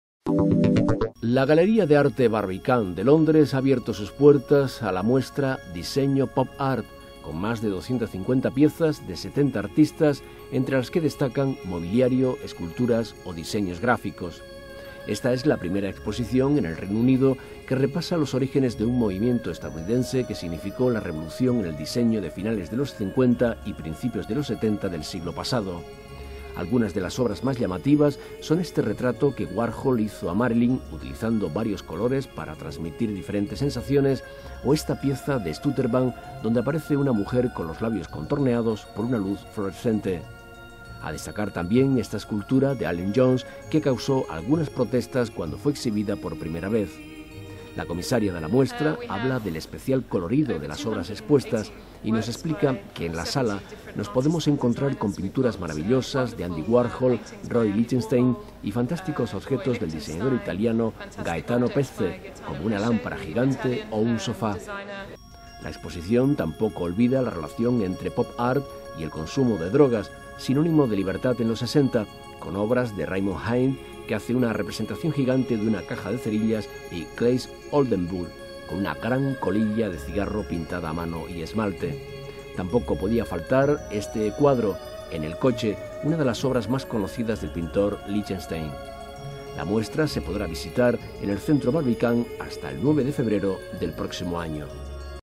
Londres repasa, por primera vez, los orígenes del pop art
Londres, oct (EFE).- (Imagen: David Palacios ).- La galería de arte Barbican de Londres presentó hoy una muestra con más de 250 piezas de la corriente "pop art", un movimiento de raíces estadounidenses que significó un cambio radical y una revolución en el diseño de postguerra.